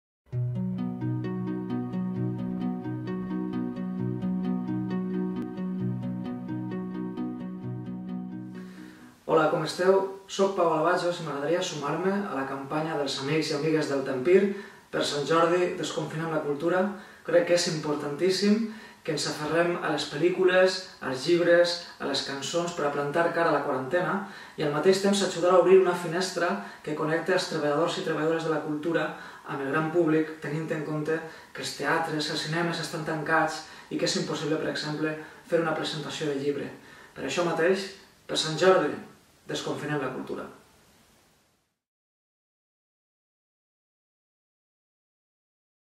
Per Sant Jordi, desconfinem la cultura! Amb Pau Alabajos.
El cantautor Pau Alabajos afirma que la música, els llibres, el cinema, etc. ens permeten afrontar la quarantena de la millor manera possible. Amb tot, comprar música i llibres en aquests moments també és una manera de donar suport a tot aquell que es dedica a fer la vida més agradable des del món cultural. Per SantJordi, desconfinem la cultura!